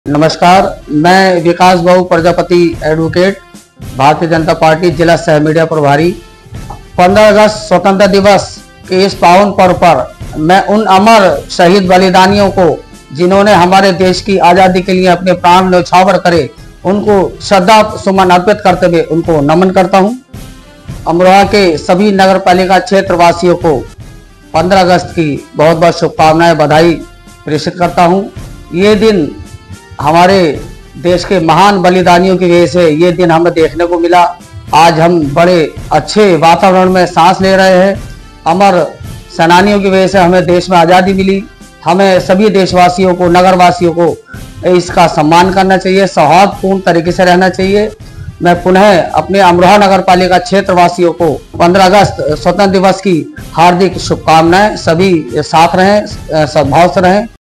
विकास बाबू प्रजापति एड. जिला सह मीडिया प्रभारी BJP की ओर से 15 अगस्त की हार्दिक बधाई शुभकामनाएंJPN7
विकास बाबू प्रजापति एड. जिला सह मीडिया प्रभारी BJP की ओर से 15 अगस्त की हार्दिक बधाई शुभकामनाएंJPN7NEWS